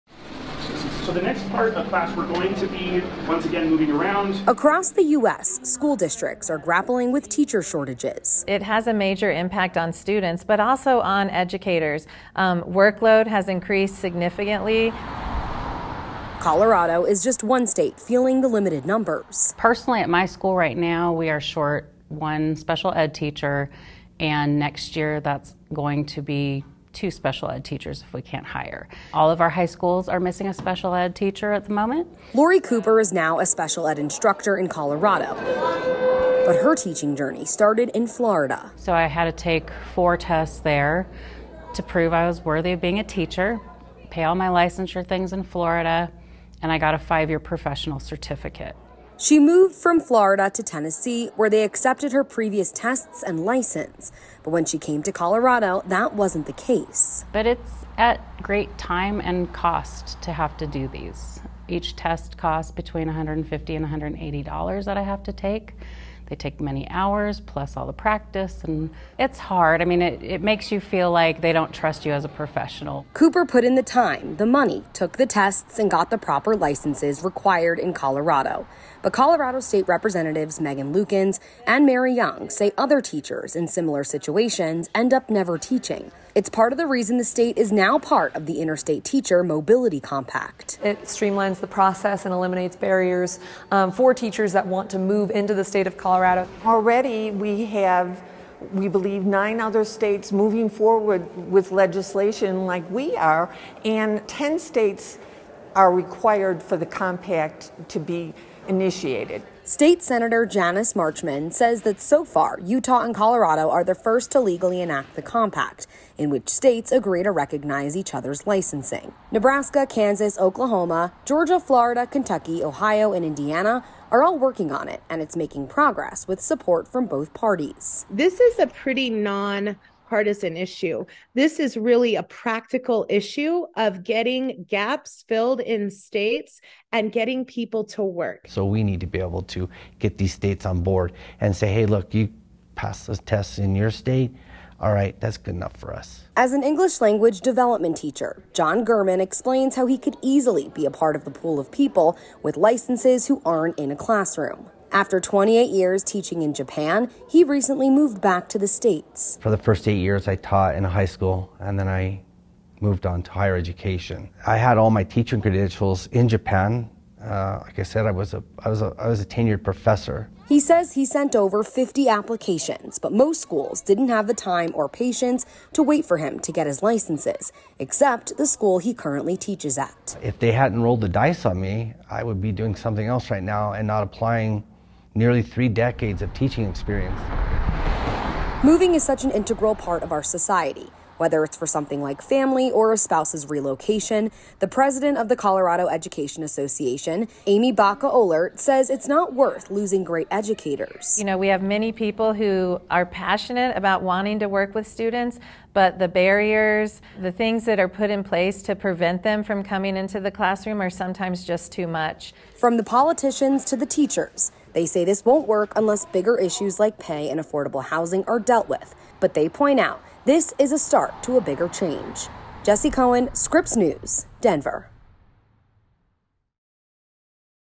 Lawmakers are trying to make it easier for teachers to move states
The complicated process of getting a teaching license after moving states is enough for some to leave the profession altogether. Lawmakers are trying to change that.